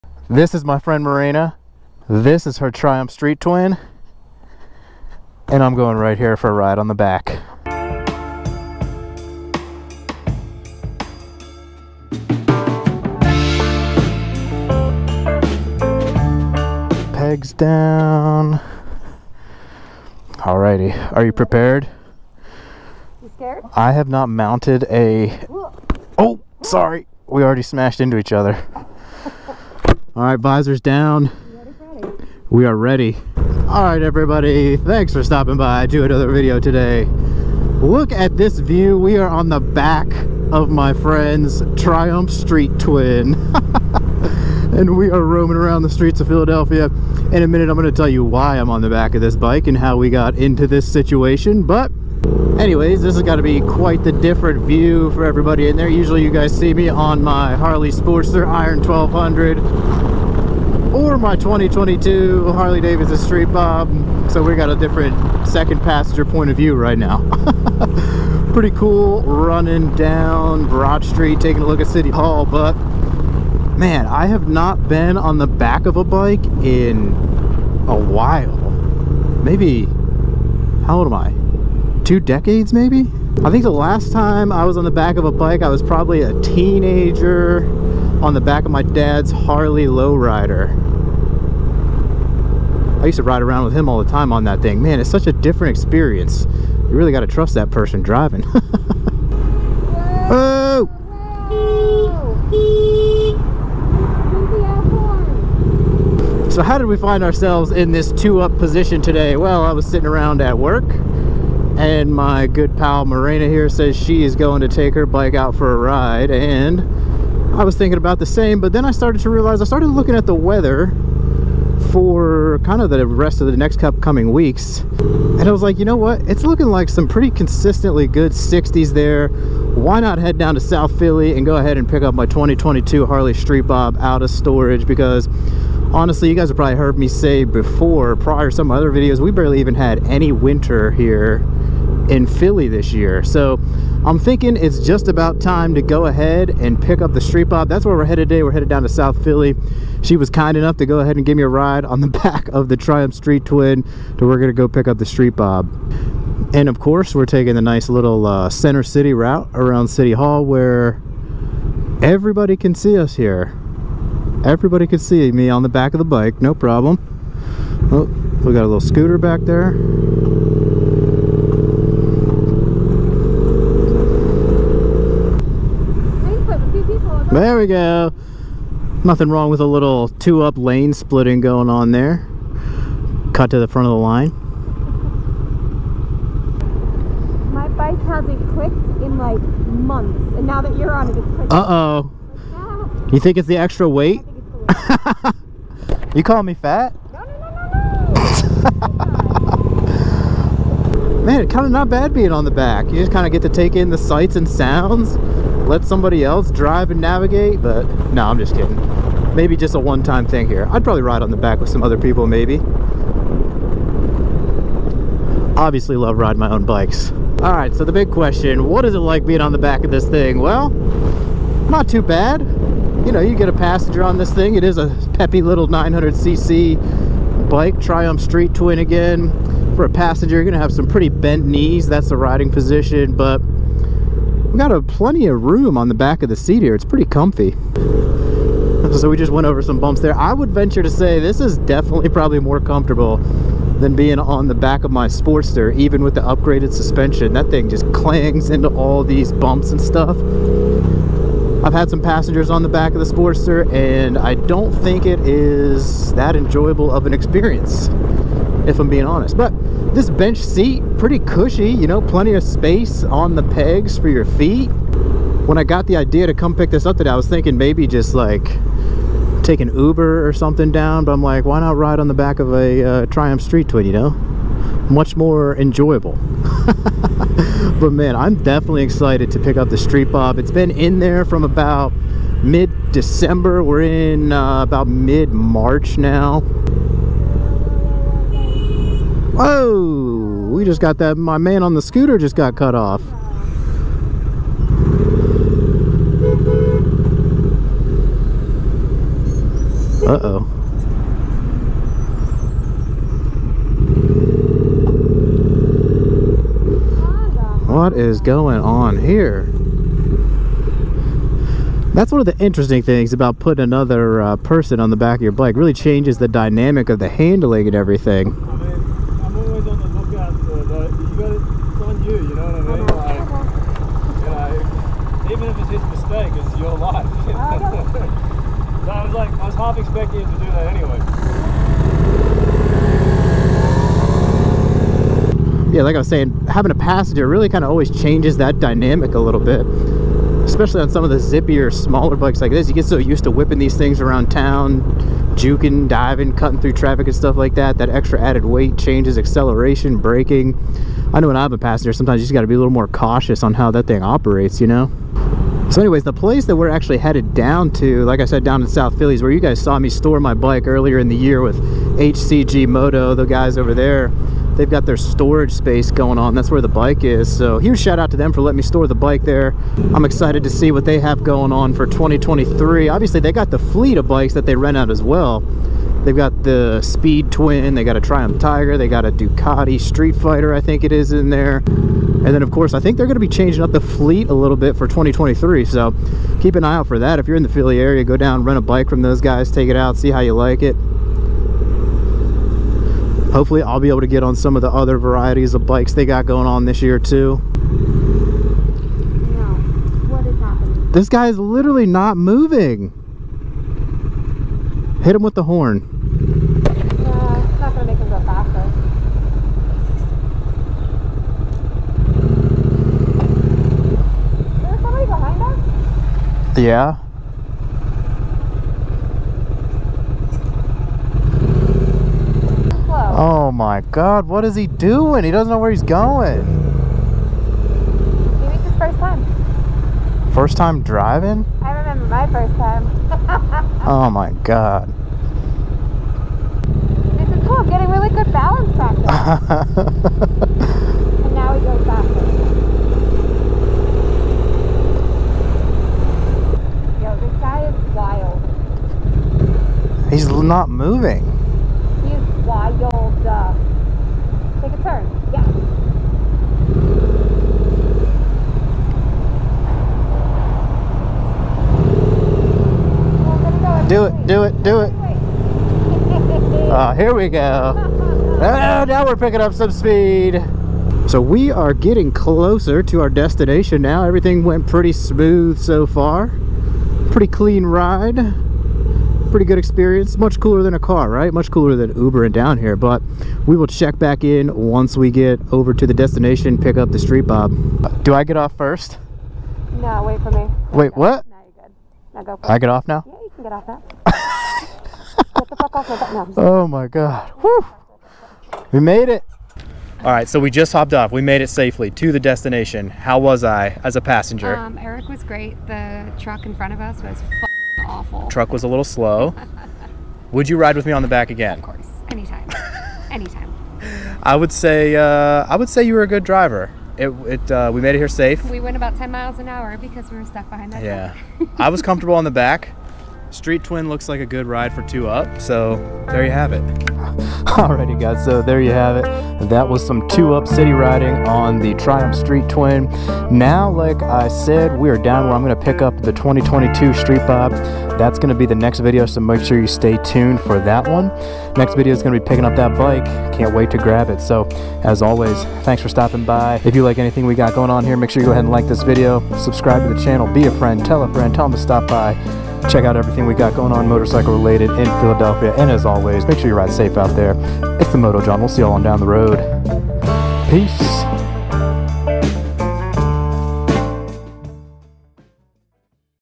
Harley Guy Rides on the BACK of HER Triumph Street Twin!!
Harley Guy Rides on the BACK of HER Triumph Street Twin!! Usually you guys see me riding around on my Harley Sportster Iron 1200 or my Street Bob but today we've got a different perspective. I'll be on the back of my friends Triumph Street Twin for a ride down to South Philly!